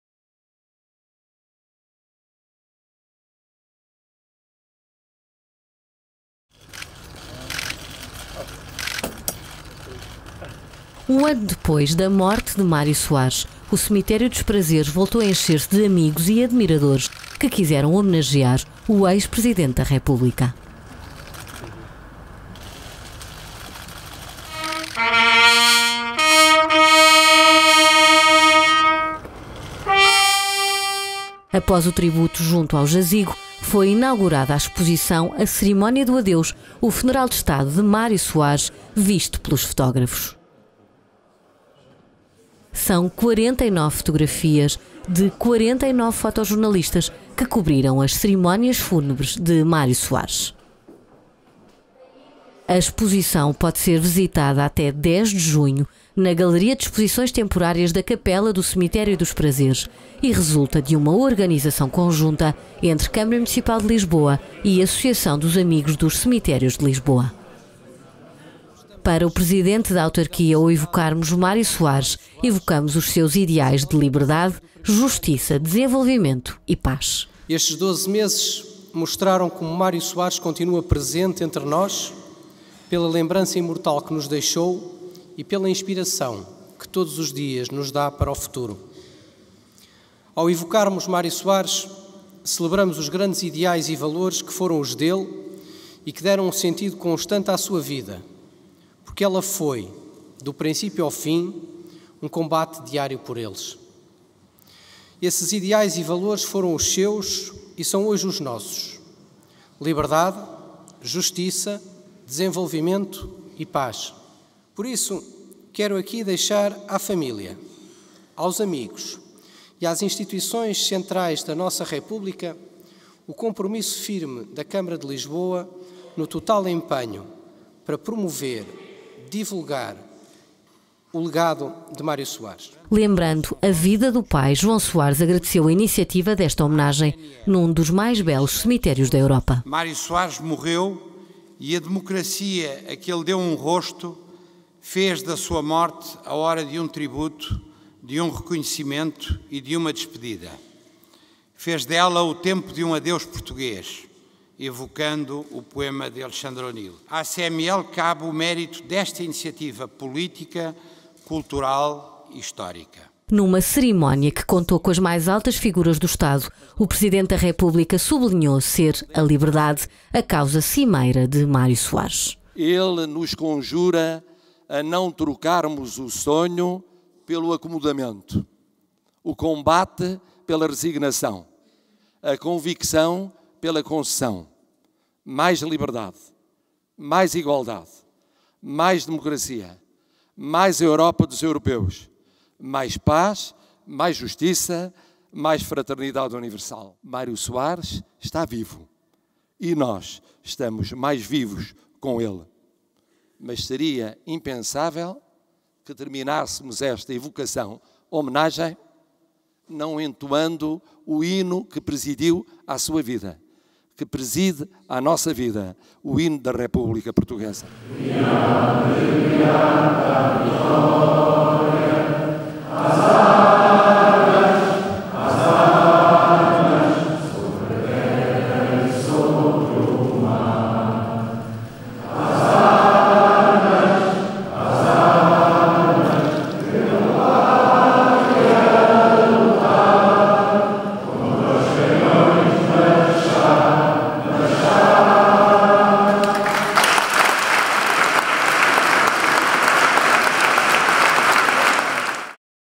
"A Cerimónia do Adeus - o funeral de Estado de Mário Soares visto pelos fotógrafos"
Decorreu no Cemitério dos Prazeres, no dia 7 de janeiro - um ano após a morte do antigo Presidente da República - a inauguração da exposição "A Cerimónia do Adeus - o funeral de Estado de Mário Soares visto pelos fotógrafos", que fica patente na nova galeria de exposições temporárias, na capela do cemitério.